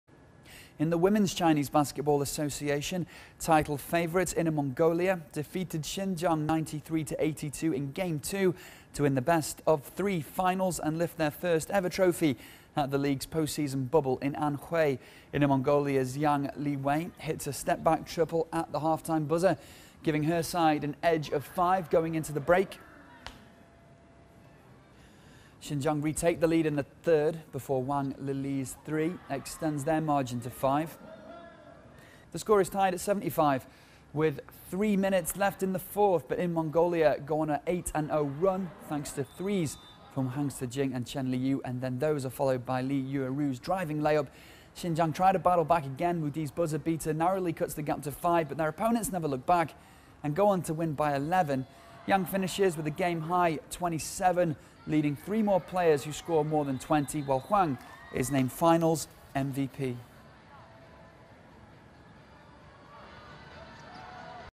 WCBA Finals - Inner Mongolia sweeps Xinjiang 2-0 to claim their first-ever title | Basketball 内蒙古女篮
In the Women's Chinese Basketball Association, title favorites Inner Mongolia defeated Xinjiang 93-82 in Game 2 to win the best-of-three finals and lift their first-ever trophy at the league's post-season bubble in Anhui. Inner Mongolia's Yang Liwei finished with a game-high 27 points, leading three players scoring more than 20, while Huang Sijing is named Finals MVP.